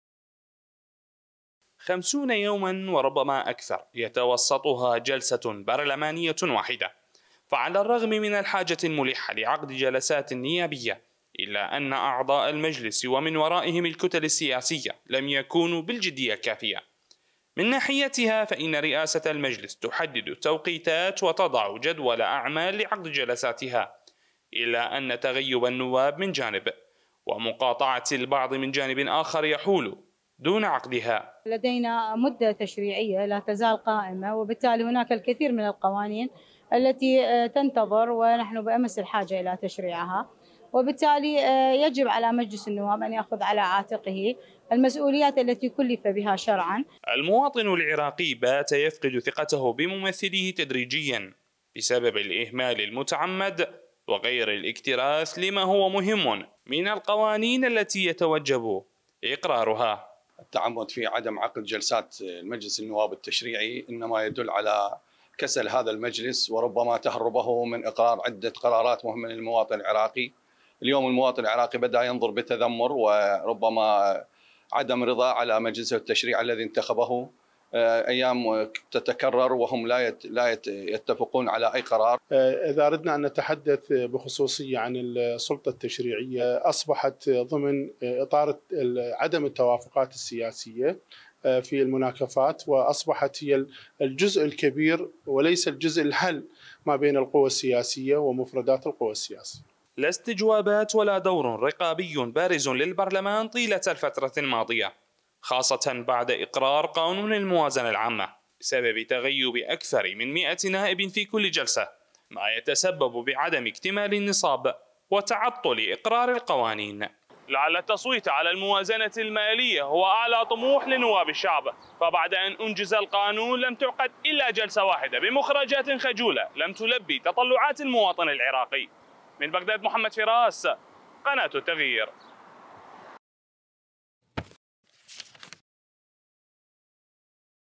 البرلمان العراقي يعطل قوانين مهمة .. والسبب | تقرير
نواب يتغيبون وآخرون يقاطعون والقوانين المهمة معطلة داخل البرلمان
وعلى الرغم من تحديد موعد جلسات البرلمان، إلا أن غياب النواب ومقاطعة بعضهم يحول دون استئناف الجلسات، الأمر الذي عطل تمرير وتعديل الكثير من القوانين المهمة